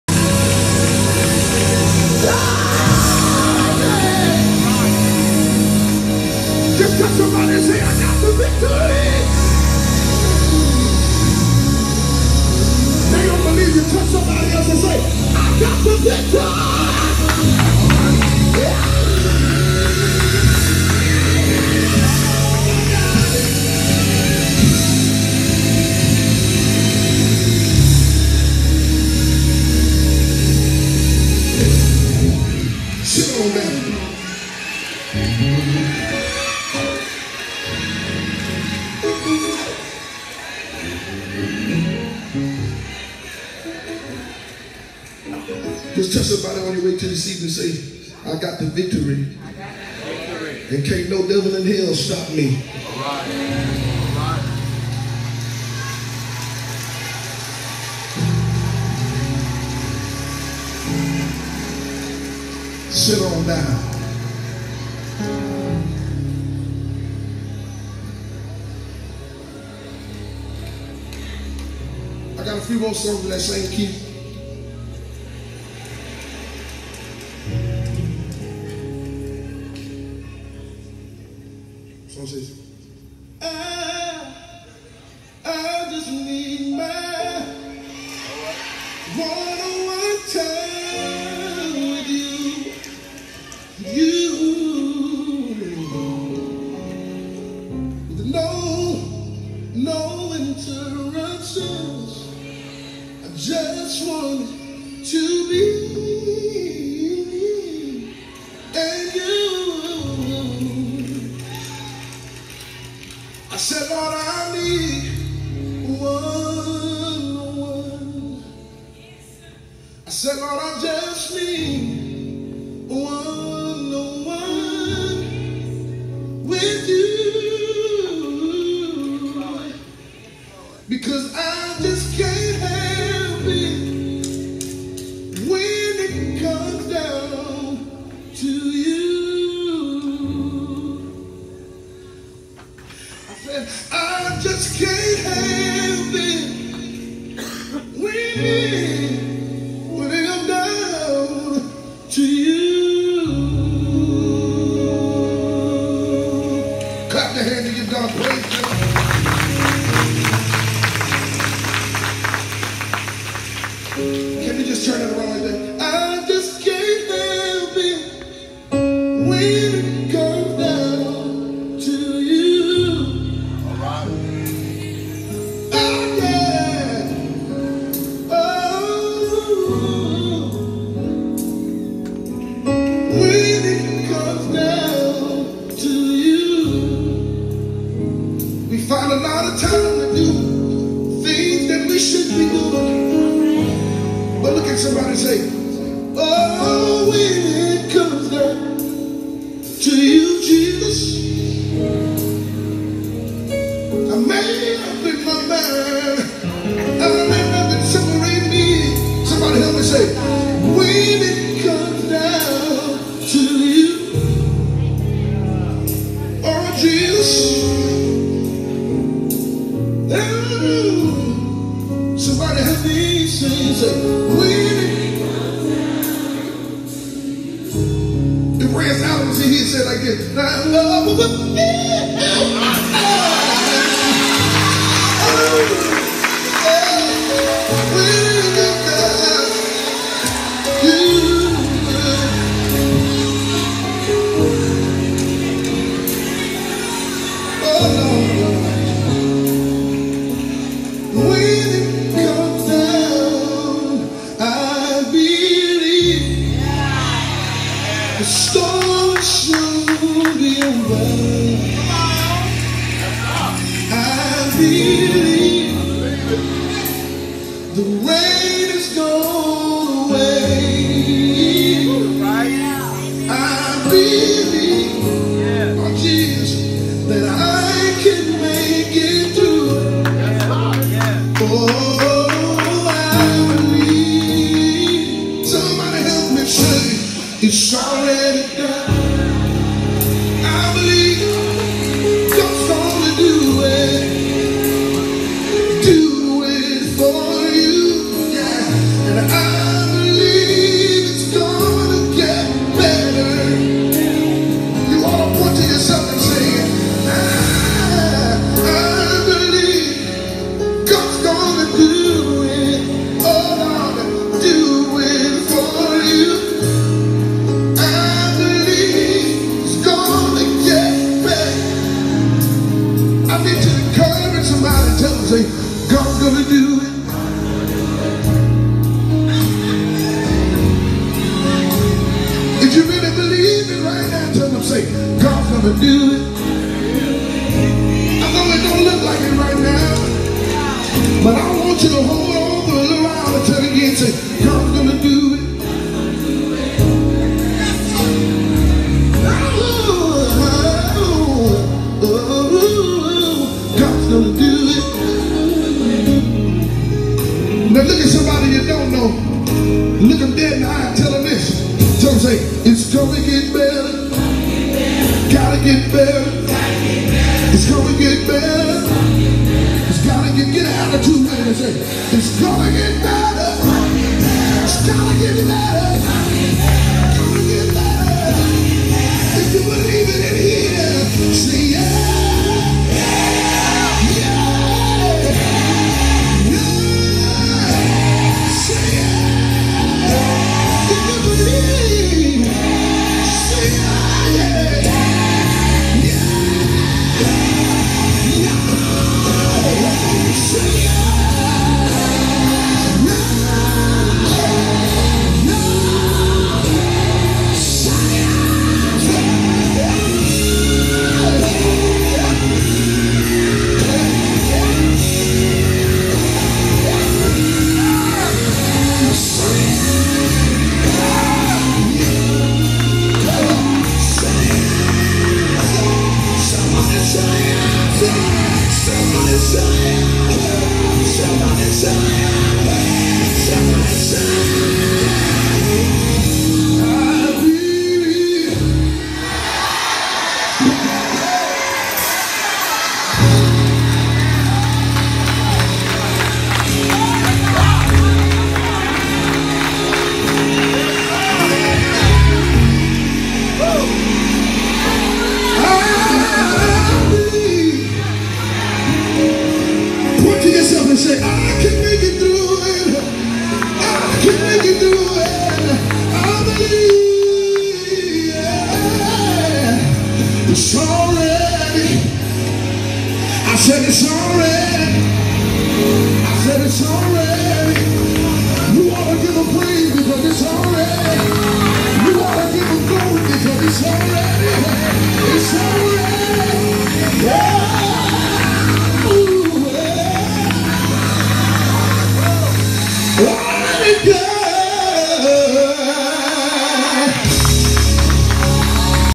KOTK Zacardi Cortez Melody Worship Mime
Kings of the King Mime Team covering Zacardi Cortez ministering at Mount Zion Baptist Church Nashville during “A Night of Expectation”
Mime performed at New City Worship in Hull, Ga.